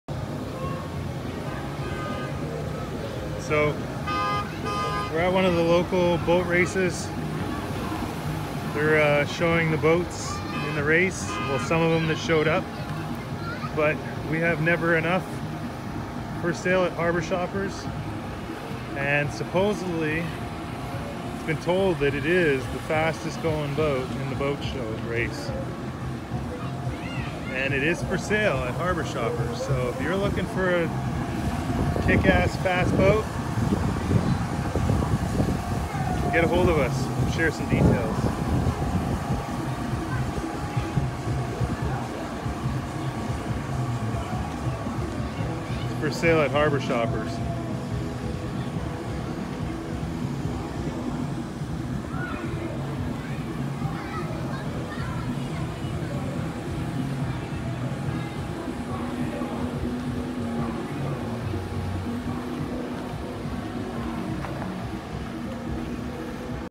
Cigarette Go Fast Racing Boat 32 for Sale
Cigarette go fast racing boat 32 for sale on Harbor Shoppers, ready to race :)